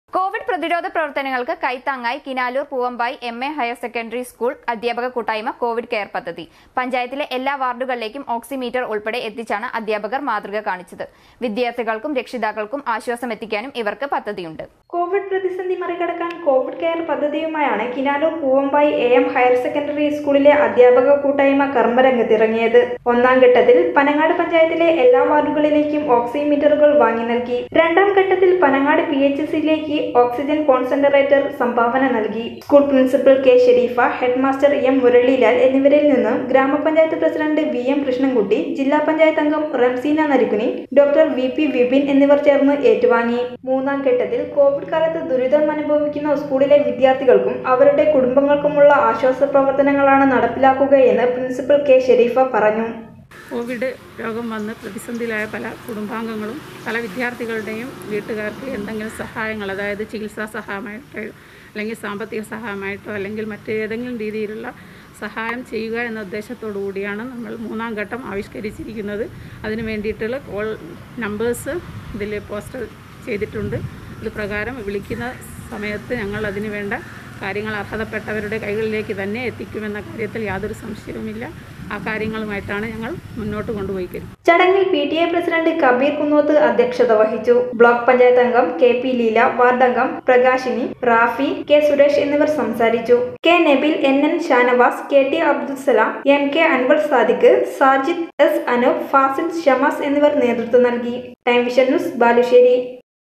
പൂവമ്പായി എ എം ഹയര്‍ സെക്കന്ററി സ്‌ക്കൂള്‍ അധ്യാപക കൂട്ടായ്മയുടെ കോവിഡ് കെയര്‍ പദ്ധതി covid care
കൊവിഡ് പ്രതിരോധ പ്രവര്‍ത്തനങ്ങള്‍ക്ക് കൈ താങ്ങായി കിനാലൂര്‍ പൂവമ്പായി എ എം ഹയര്‍ സെക്കന്ററി സ്‌ക്കൂള്‍ അധ്യാപക കൂട്ടായ്മയുടെ കോവിഡ് കെയര്‍ പദ്ധതി. പഞ്ചായത്തിലെ എല്ലാ വാര്‍ഡുകളിലേക്കും ഓക്സി മീറ്റര്‍ ഉള്‍പ്പെടെ എത്തിച്ചാണ് അധ്യാപകര്‍ മാതൃക കാണിച്ചത്. വിദ്യാര്‍ത്ഥികള്‍ക്കും രക്ഷിതാക്കളും ആശ്വാസ മെത്തിക്കാനും ഇവര്‍ക്ക് പദ്ധതിയുണ്ട്. Poovampai AM Higher Secondary School Teachers' Association Kovid Care Project